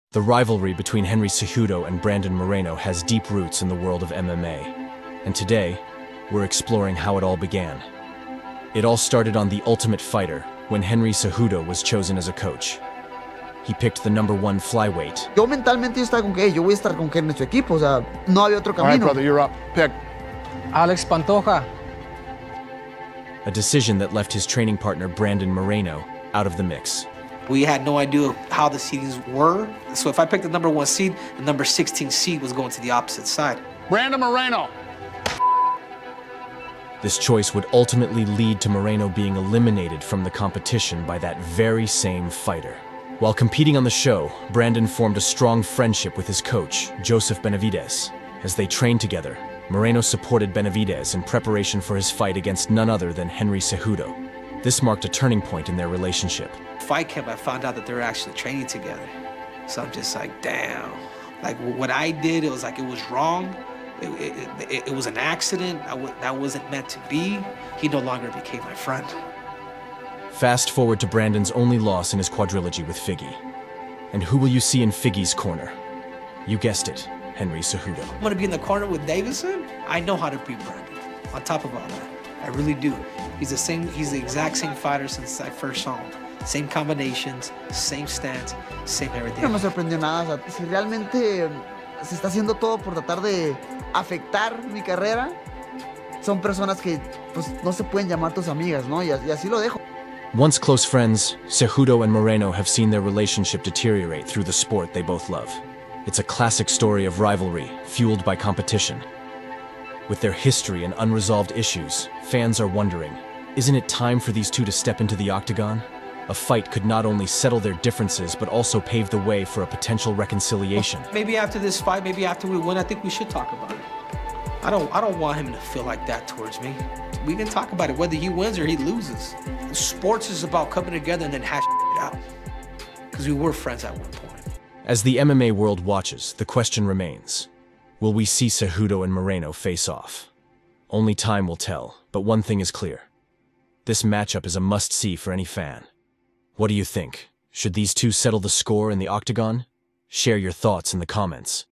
Betrayed By Your Best Friend - The Cejudo & Moreno Story
The Beef That Ended A Friendship: Brandon Moreno vs. Henry Cejudo

In this video, we dive into the intense rivalry between Brandon Moreno and Henry Cejudo, two of the most skilled fighters in the Ultimate Fighting Championship. Their beef went beyond the octagon, ultimately ending a friendship that once seemed unbreakable. From their early days as training partners to their heated exchanges in the media, we explore the events that led to their bitter feud. What was the final straw that ended their friendship? Find out in this video as we dissect the beef that overshadowed their careers in the UFC.